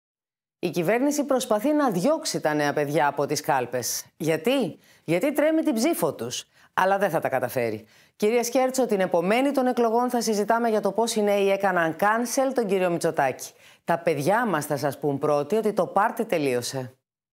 Π. Τσαπανίδου: Η κυβέρνηση τρέμει την ψήφο των νέων
Δήλωση της εκπροσώπου Τύπου του ΣΥΡΙΖΑ Προοδευτική Συμμαχία